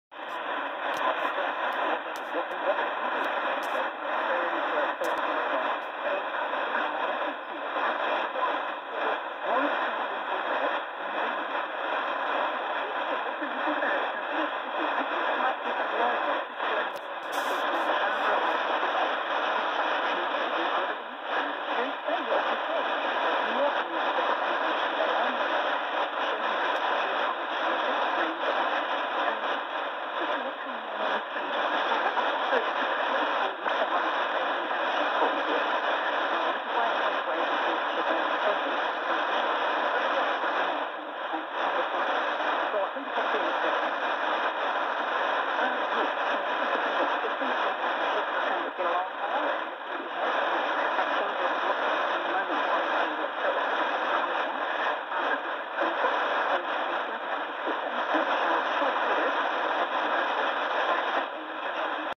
13840kHz 0447UTC Radio New Zealand International
Not as strong as the later 9700 broadcast but still better than anything I've copied at home.

Using a Roberts R861 with a Datong AD370 active antenna.